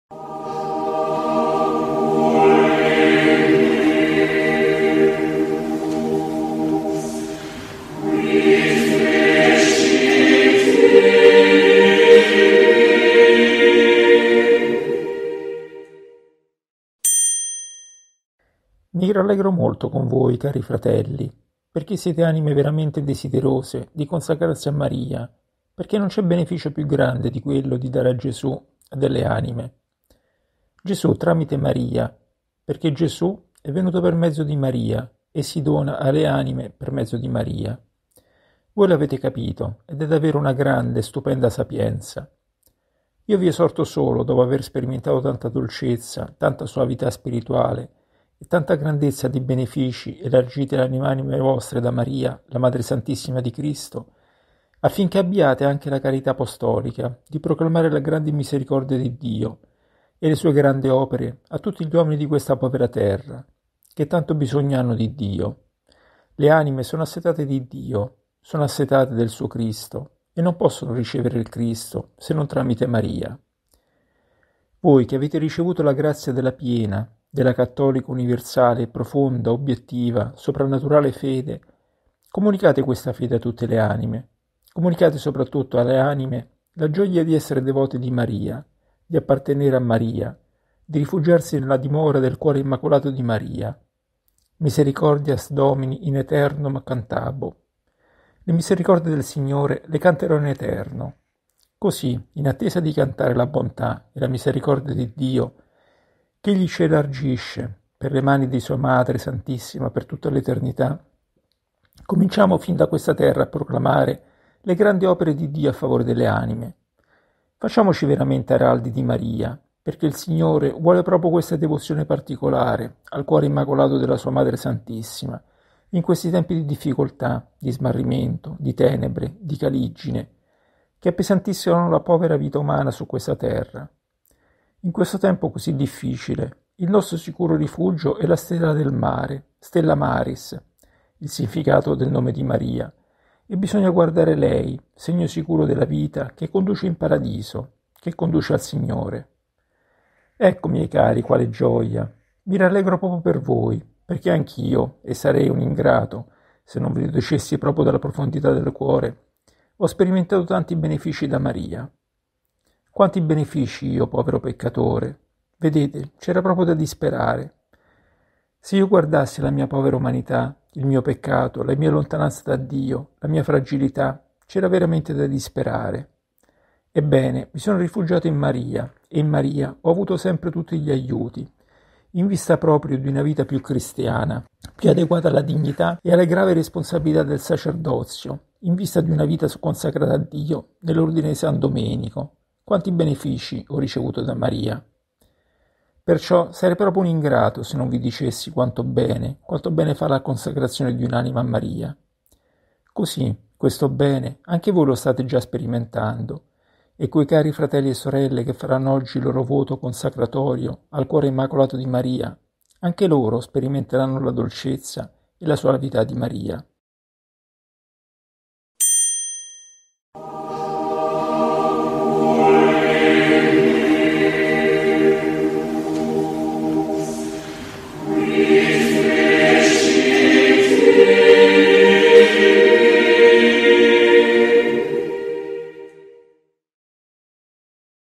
50 - Consacrazione a Maria Santissima Festa del S. Battesimo di Gesù (Parte I)
Questo testo è tratto dal libro La Beata Sempre Vergine Maria Madre di Dio. Omelie Mariane di Padre Tomas Tyn O.P.